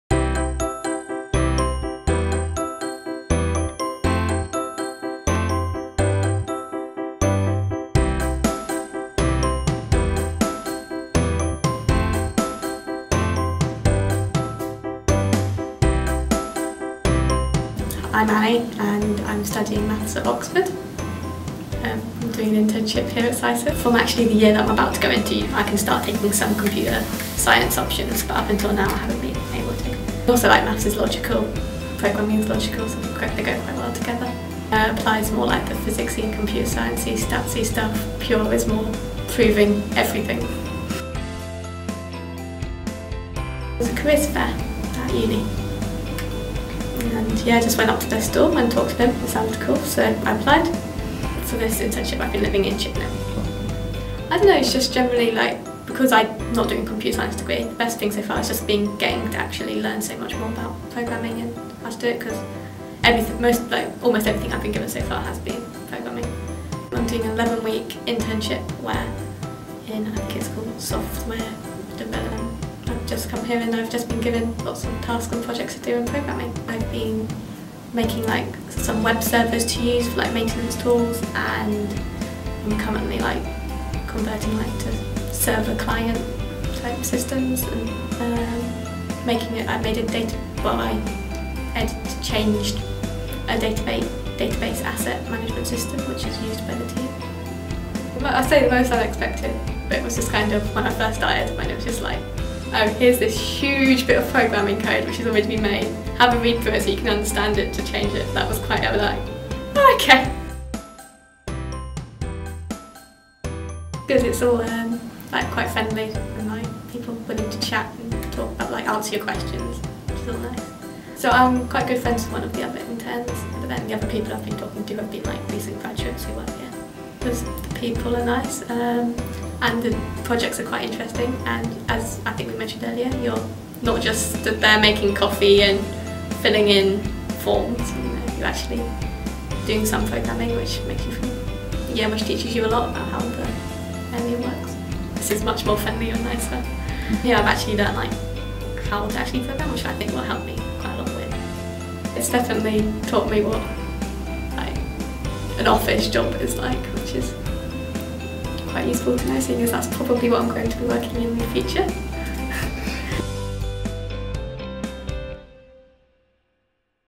SCISYS UK Internships 2017 Software Development
"The people are nice and the projects are quite interesting," that's what a recent summer intern said about working at SCISYS UK. She loved the valuable experience being part of the software development team at our Bristol and Chippenham offices.

Watch the video to see what she got up to during her time with us.